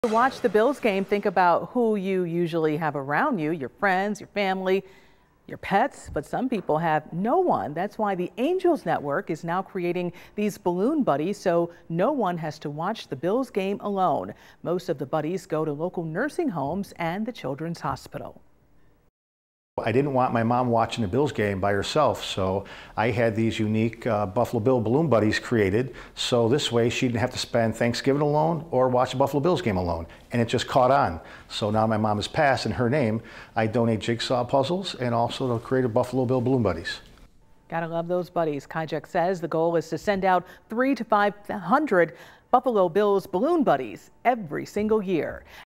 Balloon Buddies to Watch the Game with
THAT'S WHY THE "ANGELS NETWORK" IS NOW CREATING THESE BALLOON BUDDIES... SO NO ONE HAS TO WATCH THE BILLS GAME ALONE.